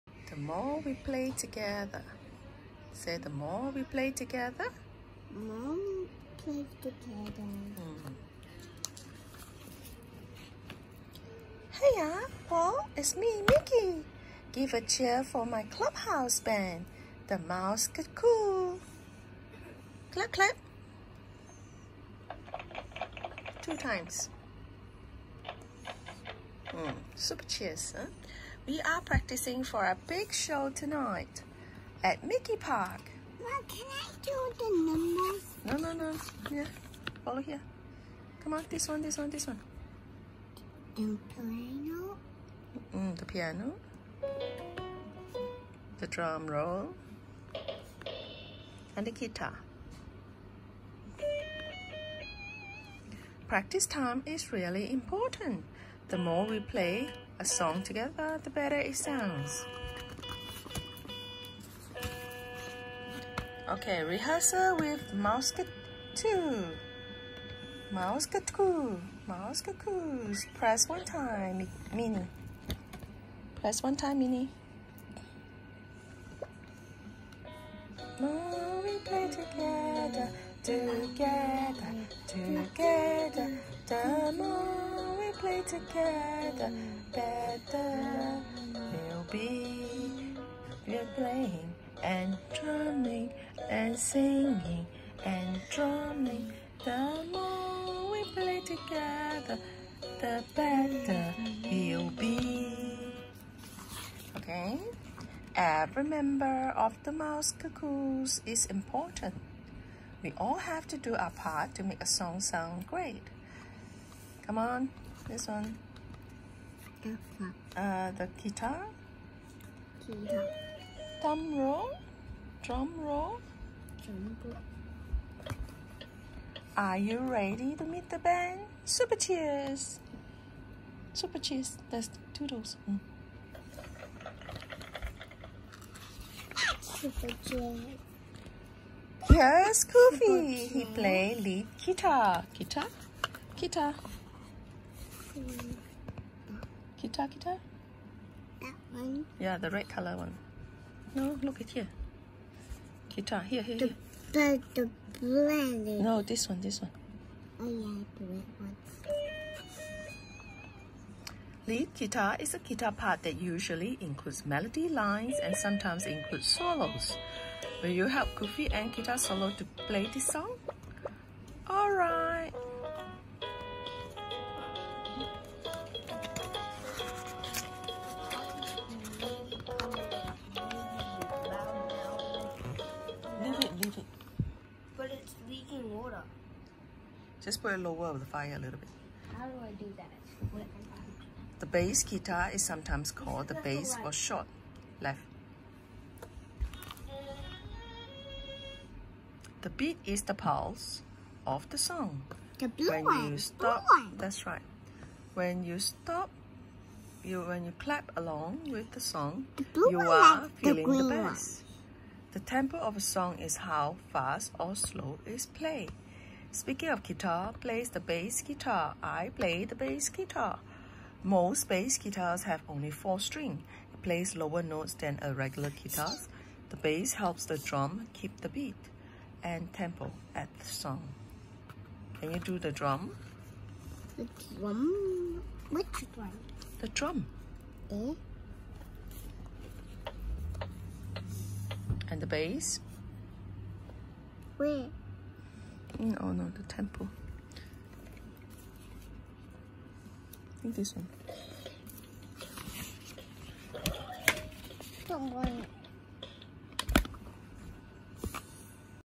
Reading The More We Play together from Mickey Mouse ClubHouse interactive reading
Doing Mickey Mouse ClubHouse interactive reading with my baby Esther again, she likes me to read to her so she can press all the buttons